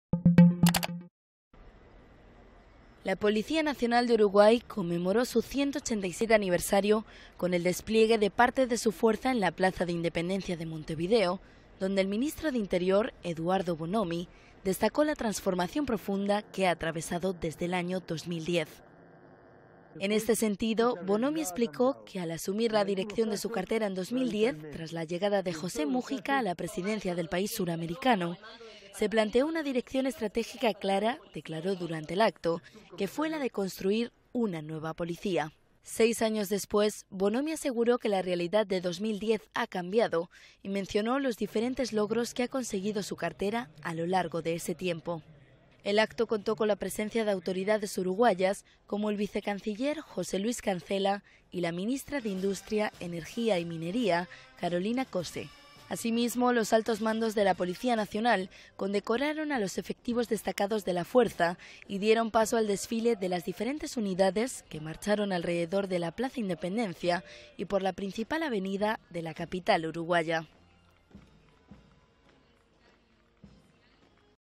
La Policía de Uruguay celebra su 187 aniversario y destaca su renovación
Montevideo (Uruguay), 19 dic (EFE).- (Imagen: María Eugenia Fernández). La Policía Nacional de Uruguay conmemoró su 187 aniversario con el despliegue de parte de su fuerza en la Plaza Independencia de Montevideo, donde el ministro del Interior, Eduardo Bonomi, destacó la "transformación profunda" que ha atravesado desde el año 2010.